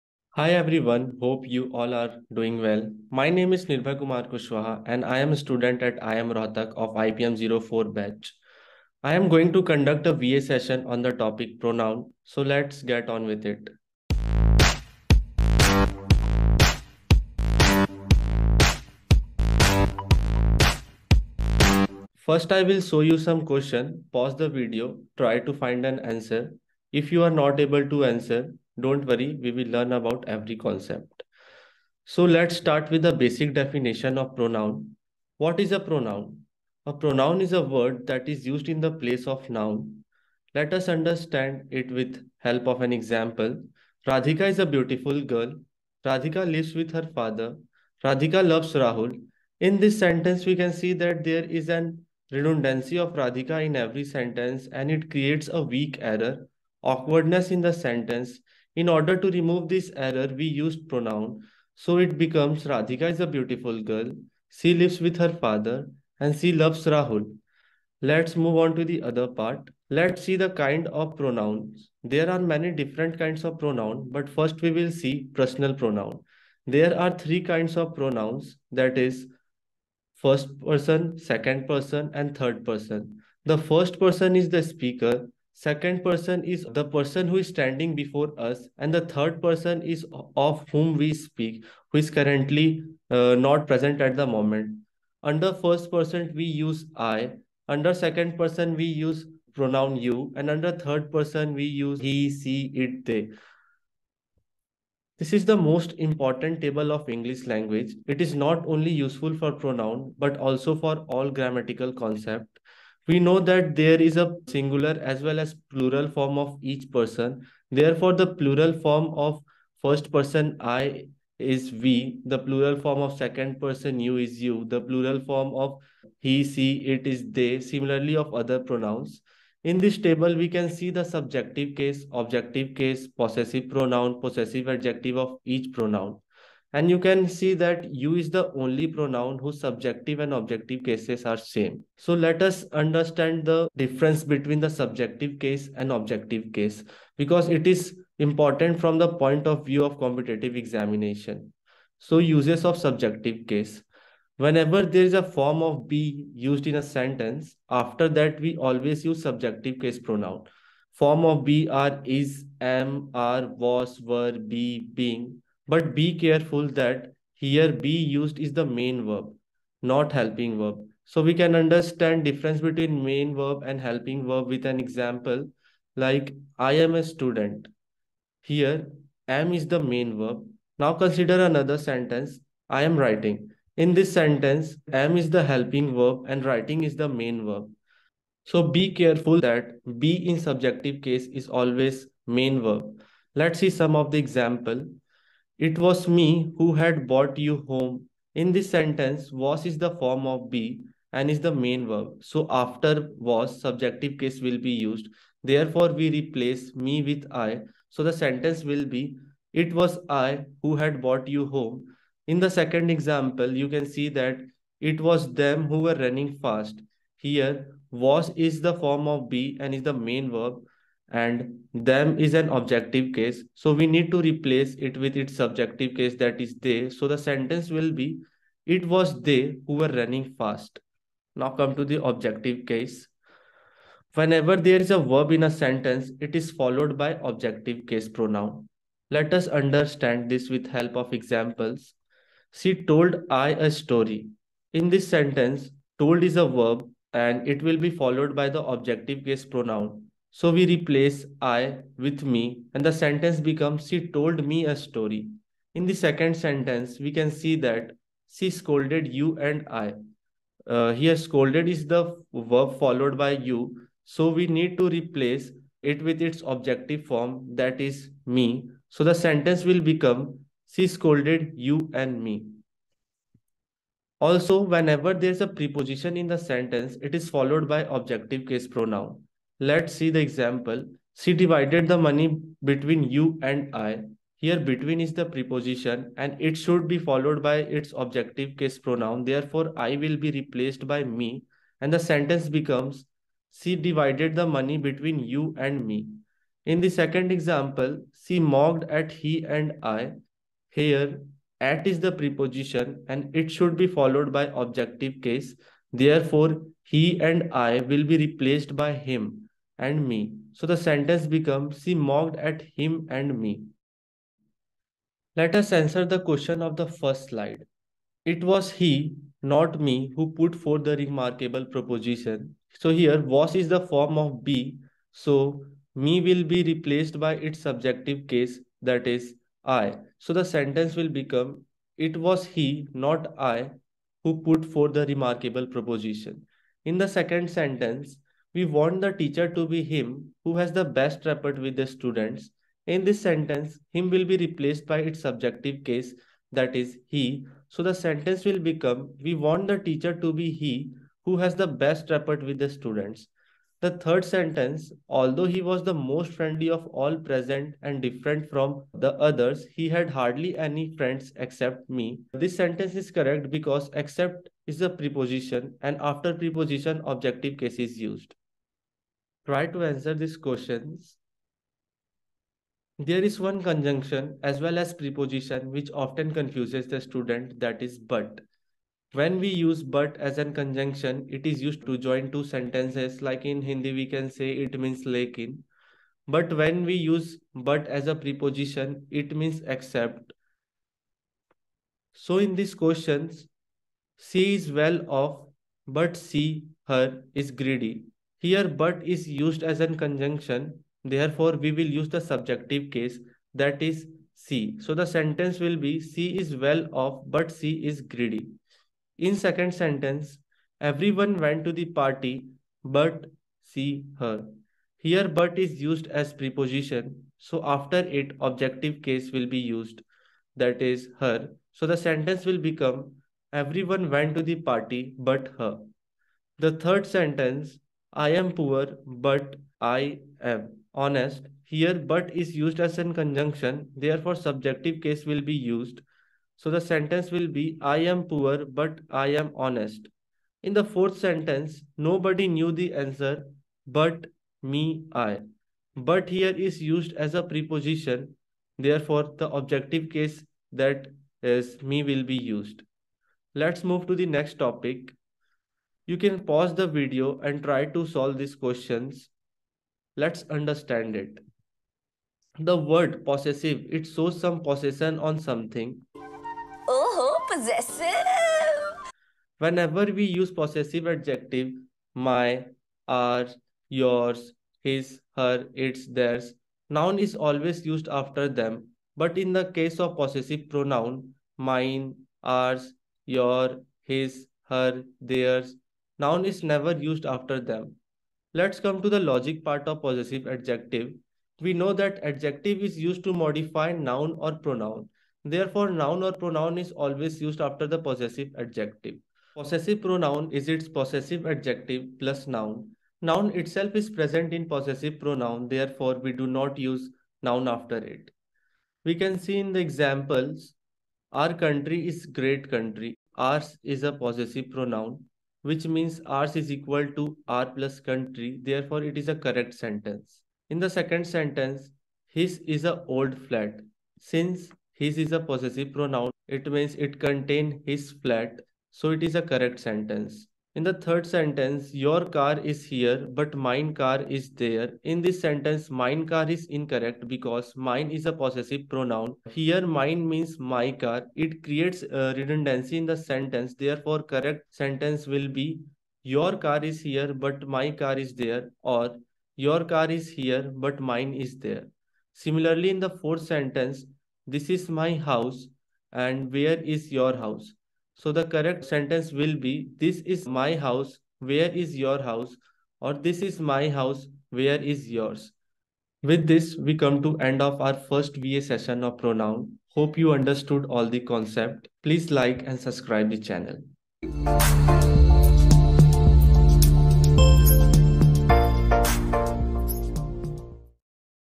Pronouns Pt.1 | Imp for IPMAT Indore | RTI Steps
Hello everyone! We are back with another video 🥳 This time, it's Pronouns

In this video, Nirbhay Khuswaha, an IPM IIM Rohtak Student of Batch 2022-27, will be explaining pronouns.
All the best for your preps! 💯💪

Timestamps:
0:00- Intro
0:32- Definition
1:10- Types of Pronouns
2:23- Subjective Case
3:51- Objective Case
4:35- Handling propositions with Pronouns
6:15- Handling sentences with 'but'
7:40- Possessive pronouns and adjectives
9:34- Outro